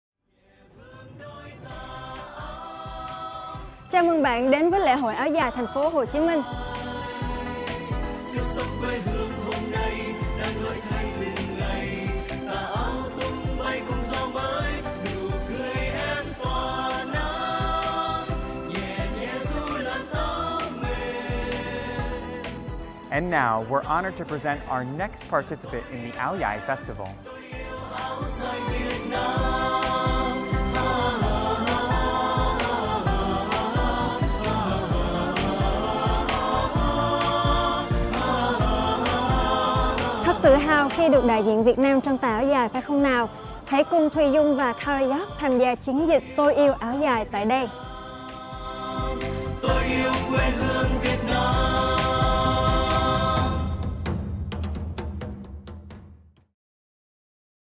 LỄ HỘI ÁO DÀI THÀNH PHỐ HỒ CHÍ MINH 2020 - PHẠM VÕ THỊ HÀ QUYÊN
Từ khi còn là cô bé con, mình luôn ước ao được mặc tà áo dài trắng thướt tha. Ngày cuối cùng của cấp ba, mình về nhà, khoá cửa phòng, tự dưng nước mắt lăn dài vì phải chia tay tà áo trắng.
Khi đi du học, hành trang của các bạn gái luôn có một tà áo dài. Tà áo giản dị, chân phương mà luôn khiến tụi mình nổi bật trong các buổi giao lưu với các bạn quốc tế.
Các bạn mình đến thăm Việt Nam, đều mong muốn được mặc lên mình tà áo dài yêu kiều của đất nước Việt Nam.
Mình yêu Áo dài, yêu Việt Nam!

Rất vui được làm quen với các bạn!
Phạm Hà Quyên